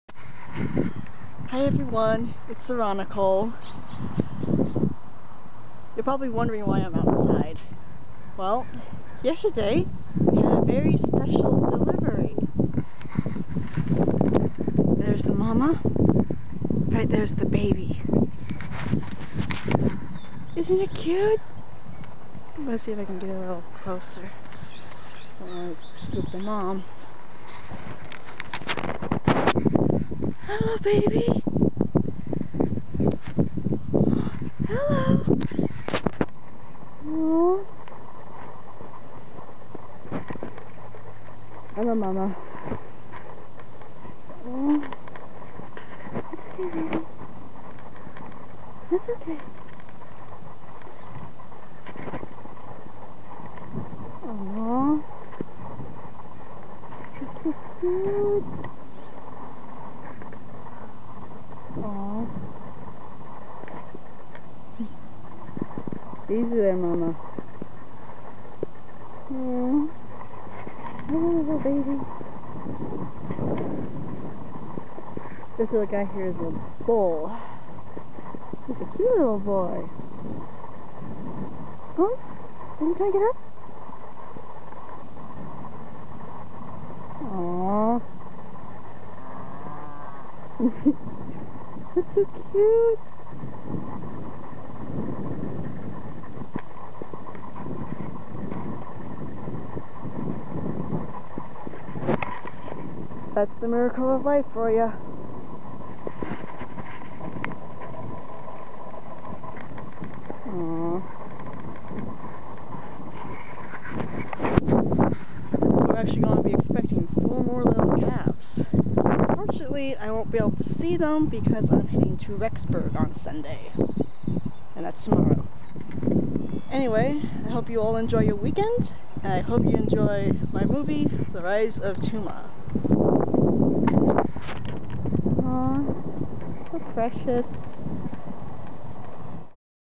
Special Delivery
Yesterday a baby cow was born. I'm expecting four more, but I won't be around to see them.